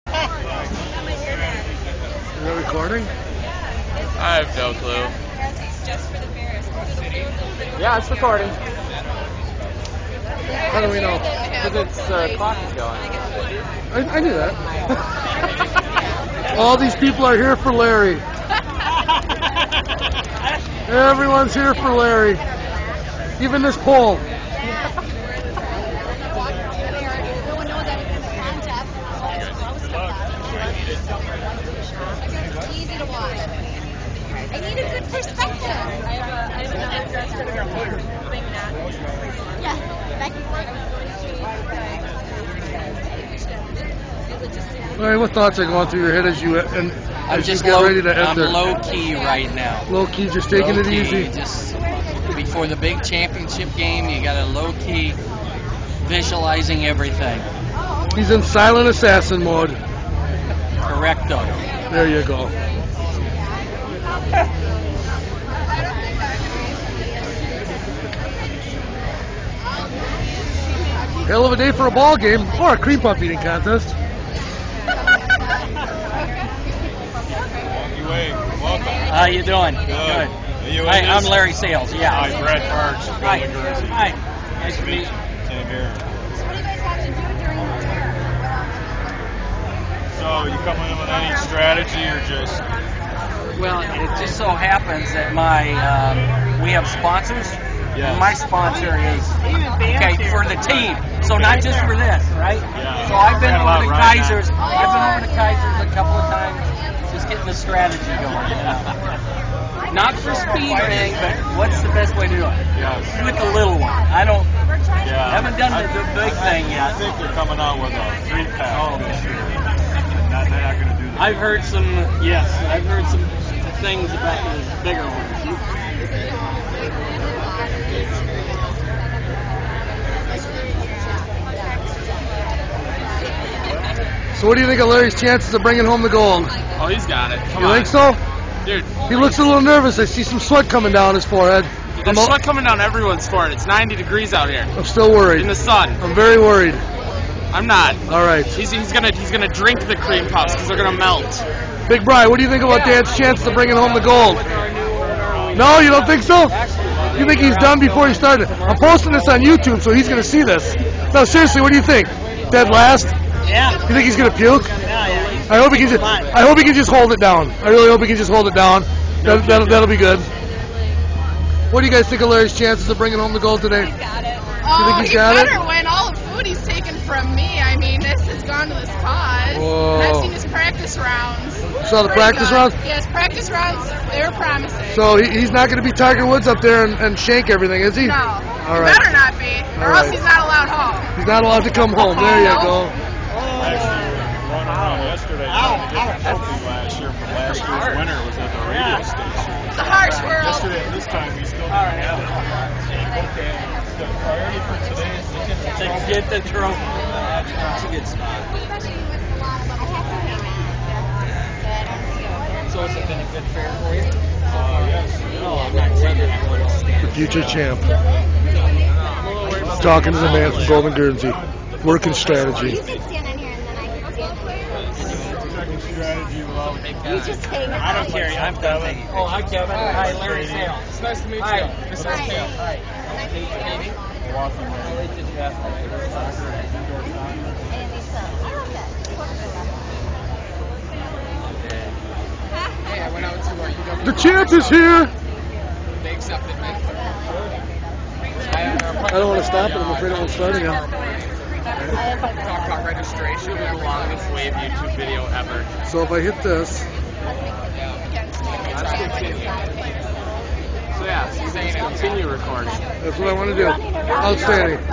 Wave Athletic Trainer Larry Sayles prepares for the Cream Puff Eating Contest
Milwaukee Wave Athletic Trainer Larry Sayles prepares for the Cream Puff Eating Contest at the 2010 Wisconsin State Fair